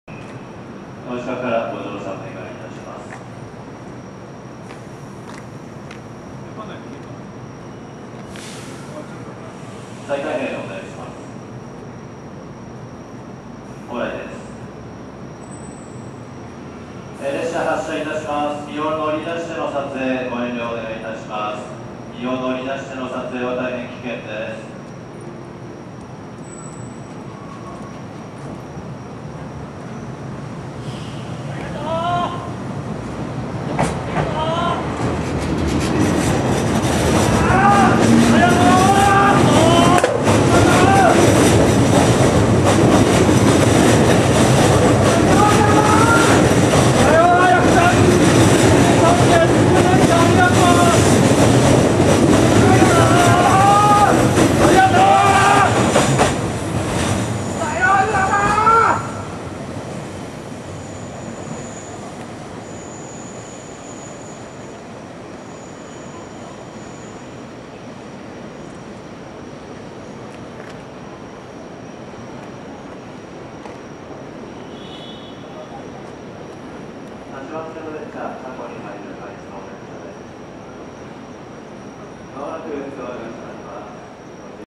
2011.8.31千葉駅 総武本線393M銚子行き113系発車シーン
2011年8月31日千葉駅を21時36分に発車する総武本線銚子行きの発車シーンです。113系定期運用終了前日とあってかなり人がいます（8月末までの運転と発表されていたため）。ありがとうコールも罵声並に周囲にひびきわたっていました。千葉都市モノレールも並走していました。